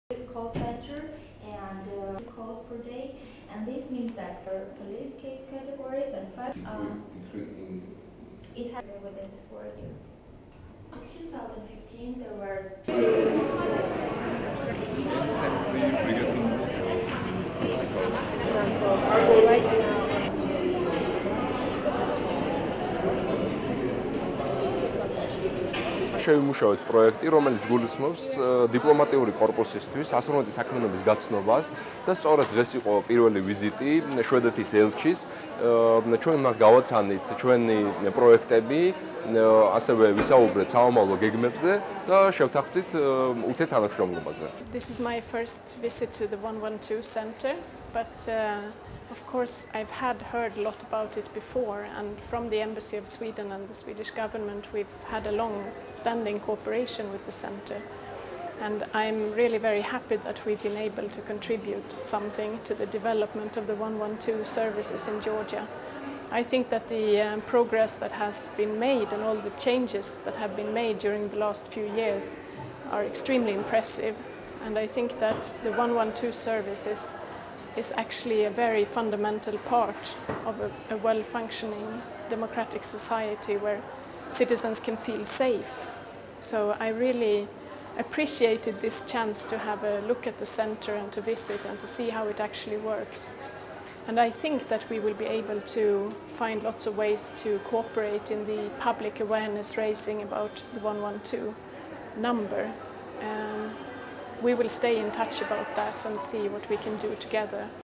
112-ის საინფორმაციო კამპანიაში შვედეთის საელჩო ჩაერთო
შსს-ს გადაუდებელი დახმარების ოპერატიული მართვის ცენტრ „112“-ს შვედეთის საგანგებო და სრულუფლებიანი ელჩი საქართველოში, მარტინა ქვიქი ესტუმრა.